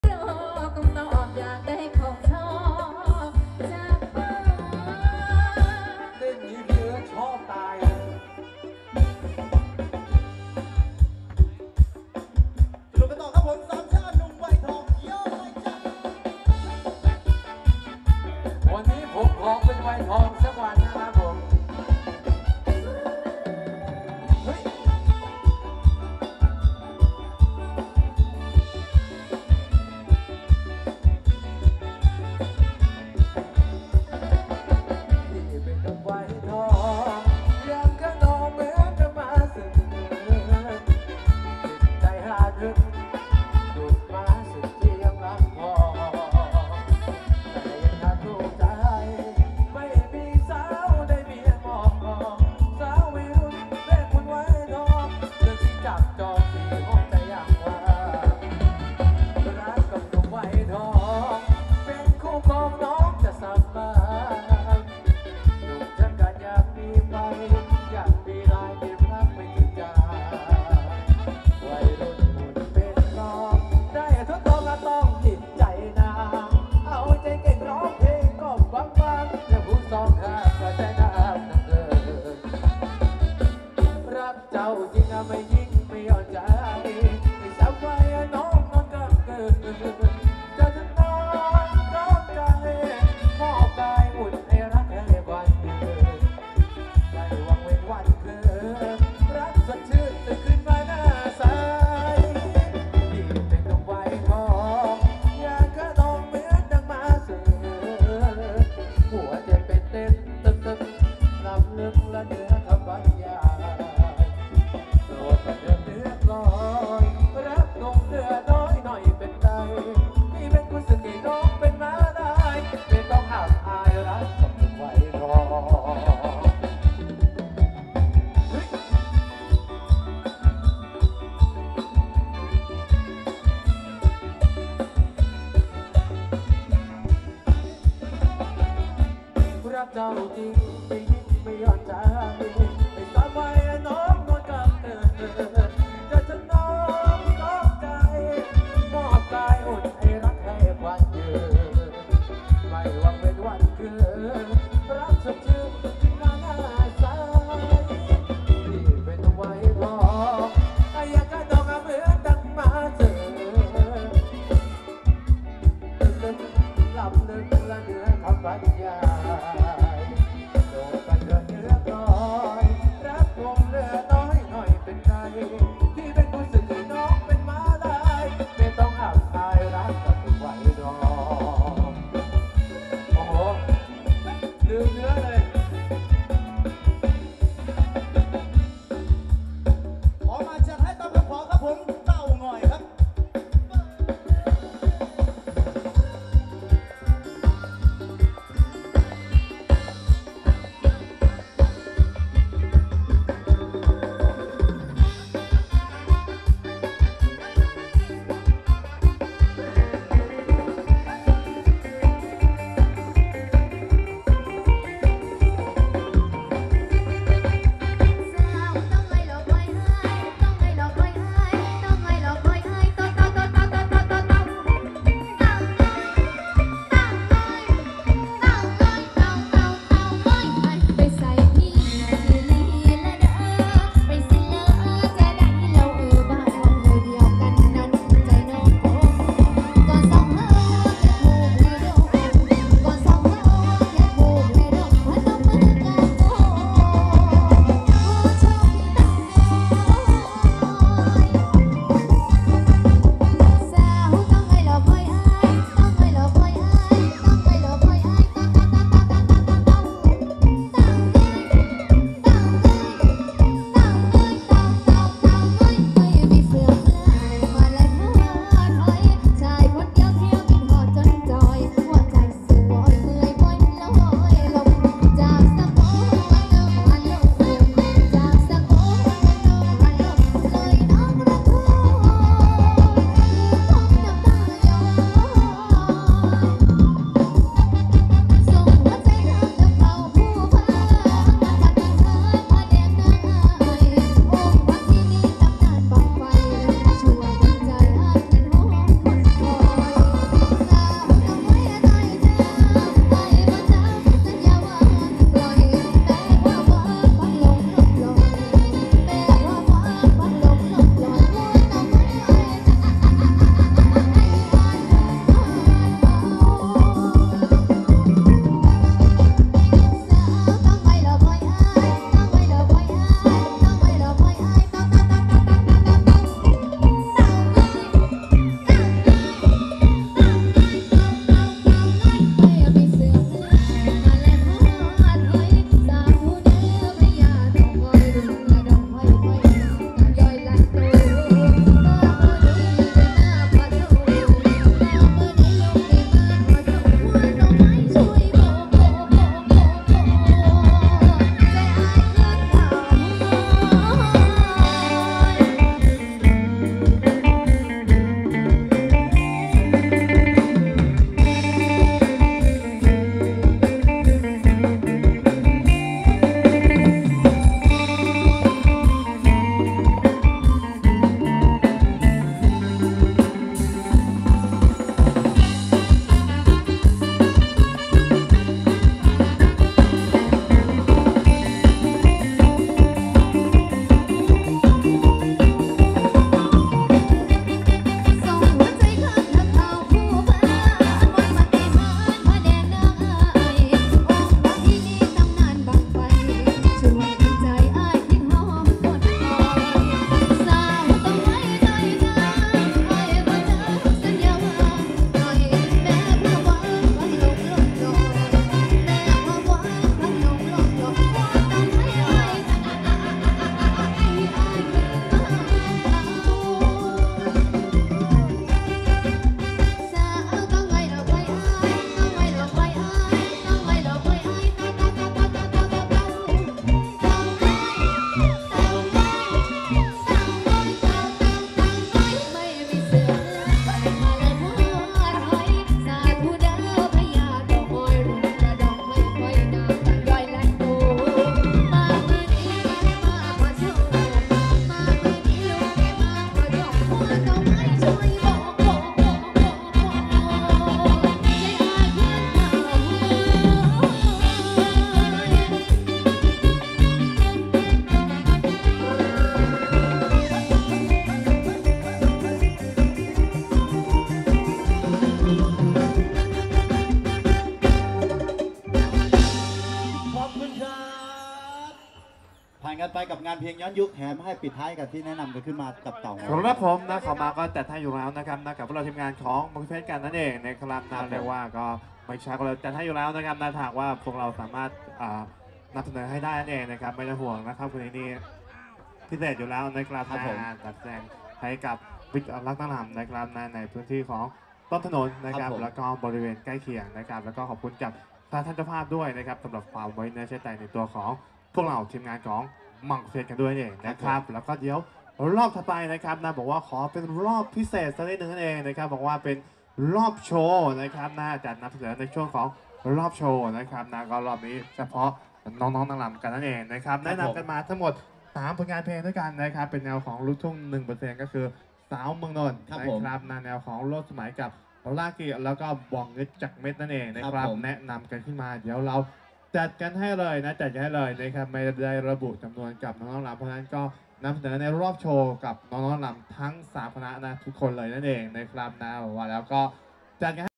[CANON]【PhetchaburiDance Workout for weight loss】รำวงเพชรบุรี✿Health❤Kawaii❀หนุ่มวัยทอง✿
name of Dancer team : Pornprommuangphet,Phetsopha,Jandara
name of Music Band : Mungkoodphet
Show location : Bantontanon,Phetchaburi Thailand